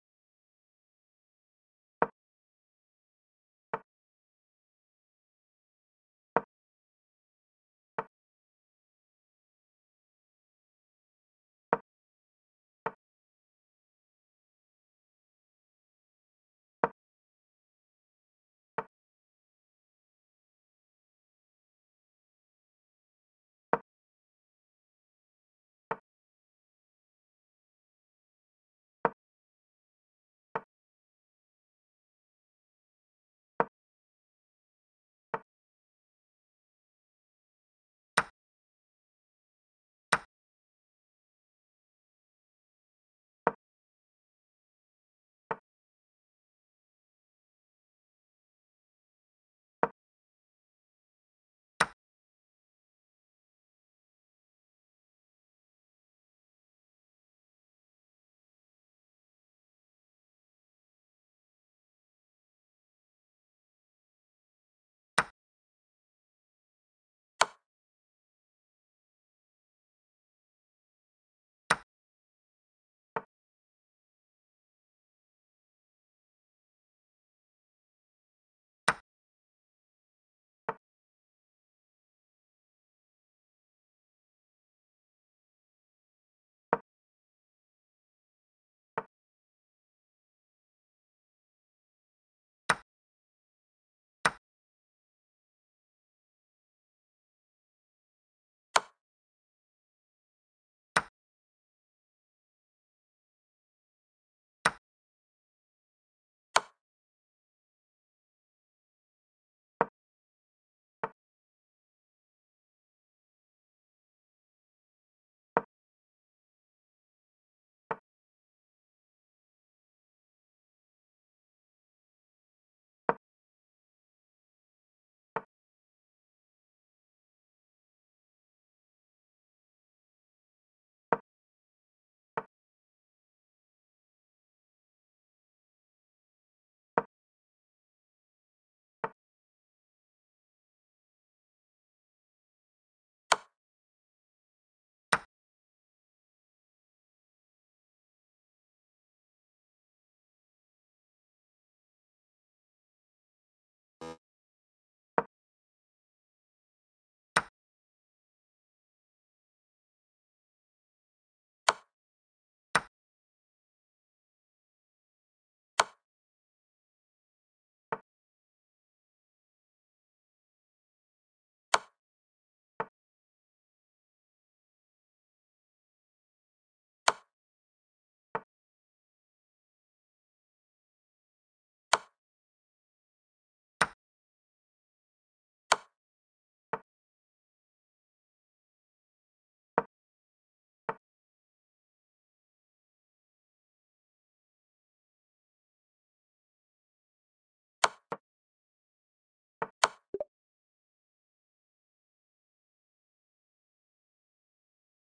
Defeating Luk.AI!
Defeating all chess bots until I beat all. Today ,we have Luk.AI!